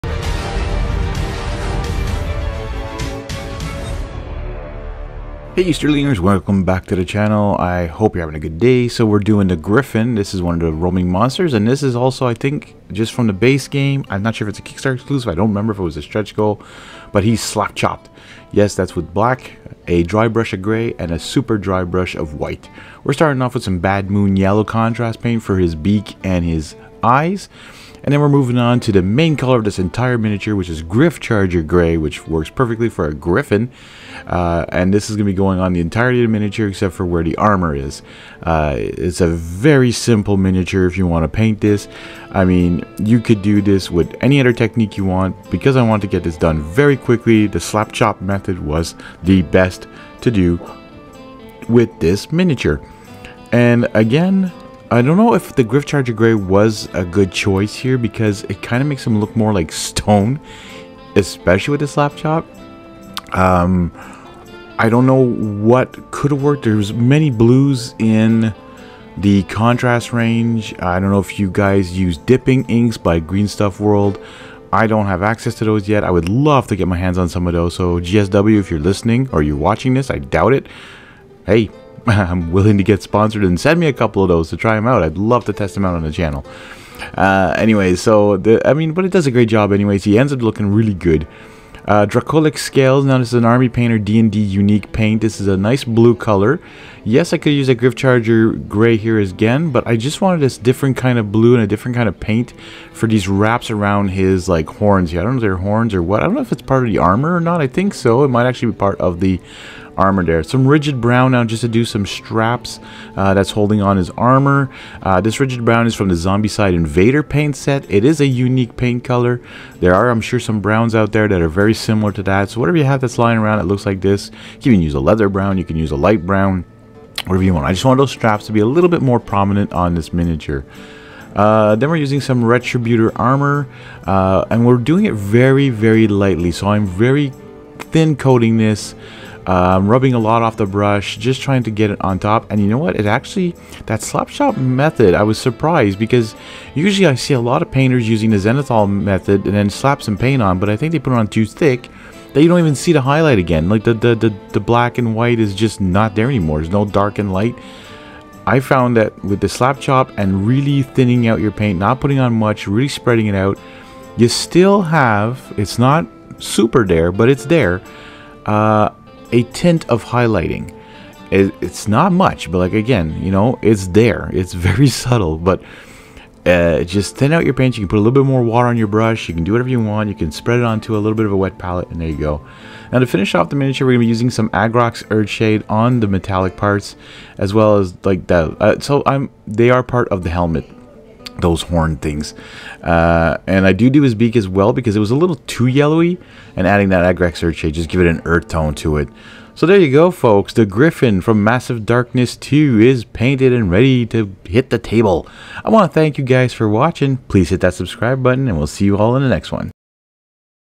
Slap Chop Painting Massive Darkness 2 Roaming Monsters - The Gryphon
Even bigger miniatures can benefit from the "slapchop" method of zenithal highlighting. Primed with Matt Black primer and then given a heavy dry brush of Army Painter's gamemaster dungeon base grey and then a top highlight of their dungeon effects white. Toss on some contrast paints and you're ready to go.
In this video I take this dull grey miniature and turn it into an intimidating, painted miniature with very little effort.

Spend less time painting and more time gaming!